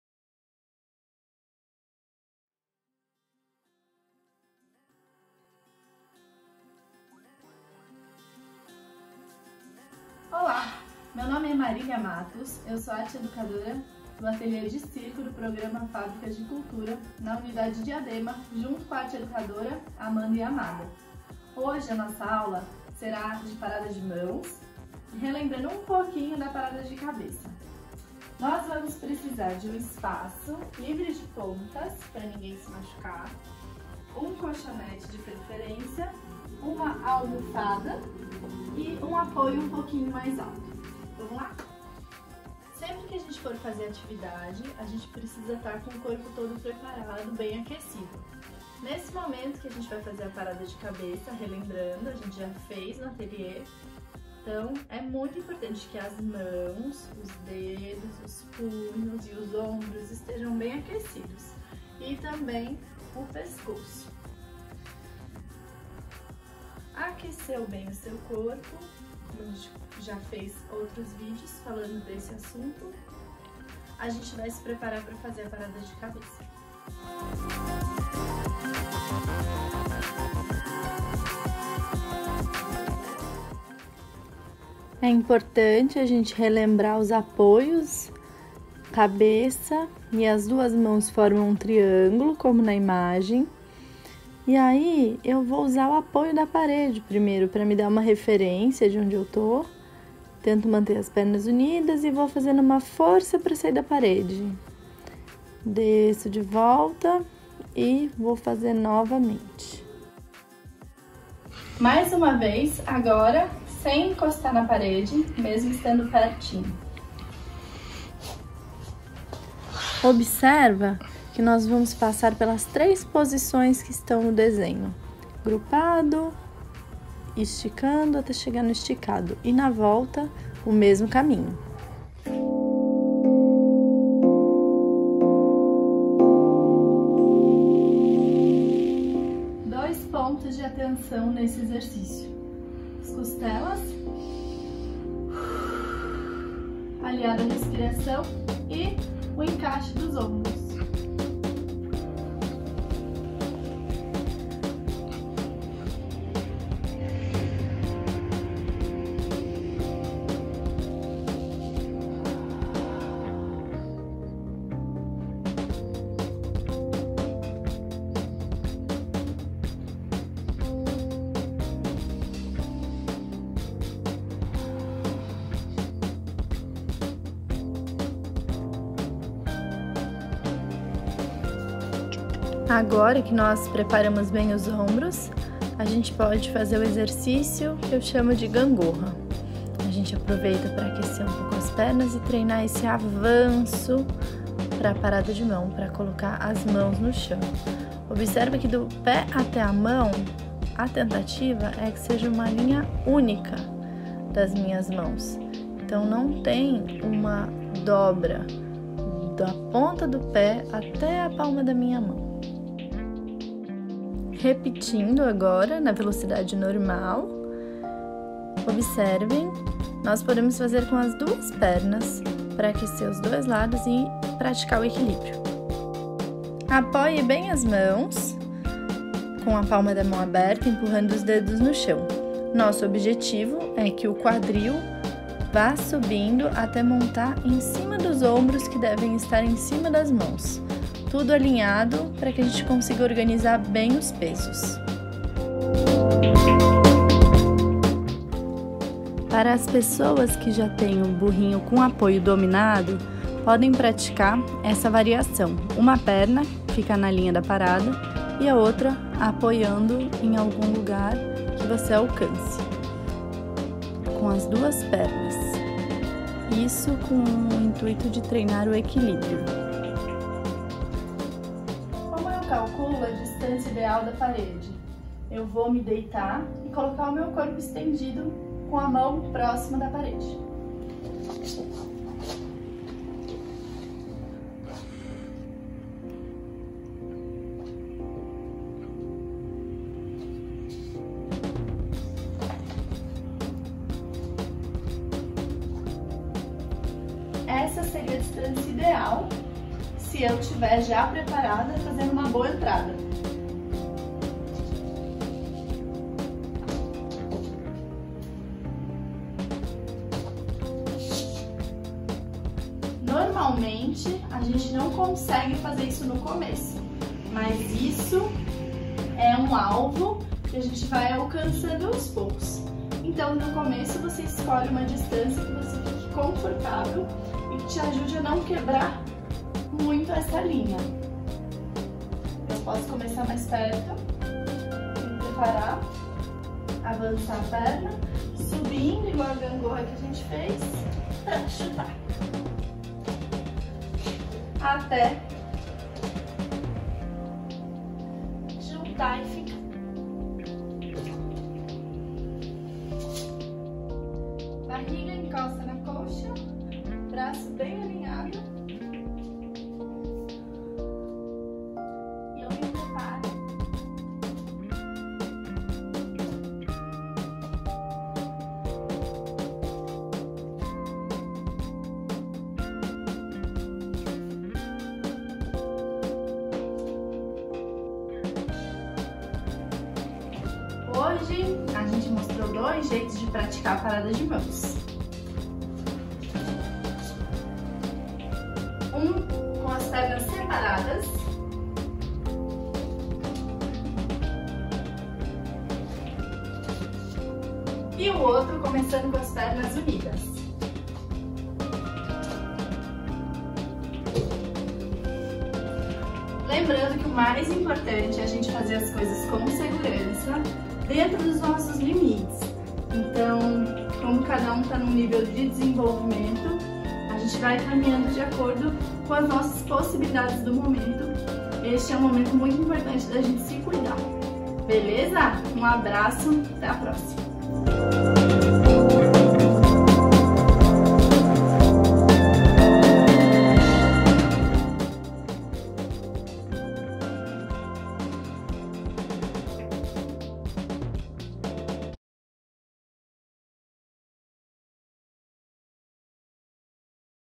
Ateliê de Circo: Acrobacias, Malabares e Números Aéreos
Videoaula com dicas para fazer parada de mão. Um auxilio para a leitura compartilhada com a turma - manual de parada de mão da FEDEC.